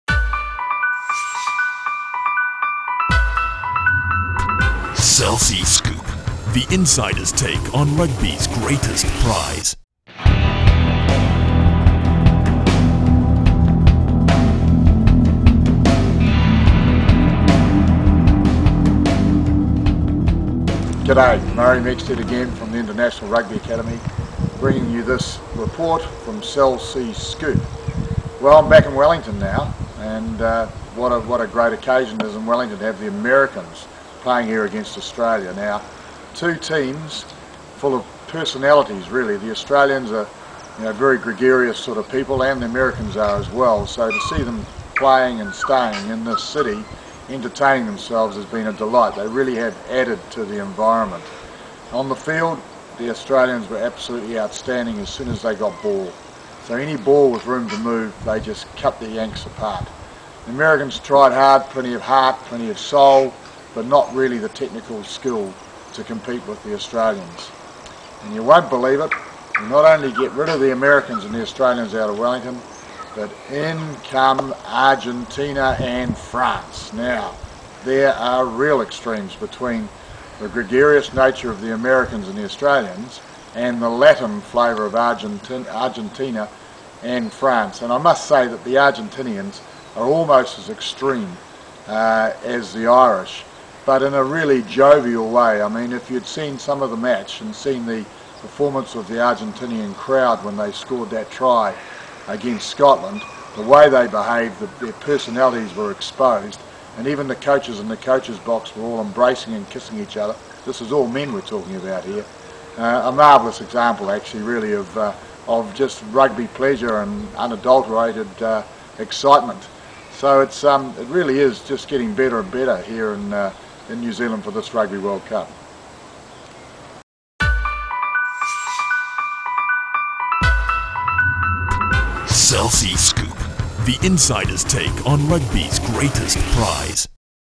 The Internationals: Murray Mexted - Men Kissing Men
Former New Zealand great and professional rugby commentator and personality Murray Mexted talks about the scenes in the coaches booth after Argentina's last gasp win over Scotland. #3#